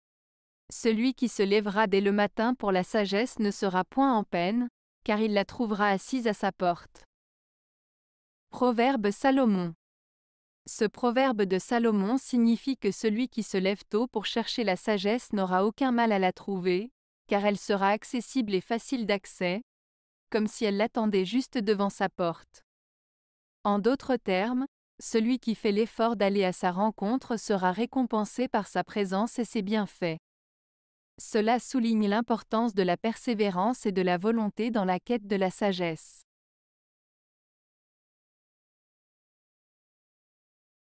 Proverbe Salomon - Celui qui se lèvera dès le matin pour la sagesse ne sera point en peine, car il l
Ce proverbe de Salomon signifie que celui qui se lève tôt pour chercher la sagesse n'aura aucun mal à la trouver, car elle sera accessible et facile d'accès, comme si elle l'attendait juste devant sa porte. En d'autres termes, celui qui fait l'effort d'aller à sa rencontre sera récompensé par sa présence et ses bienfaits. Cela souligne l'importance de la persévérance et de la volonté dans la quête de la sagesse.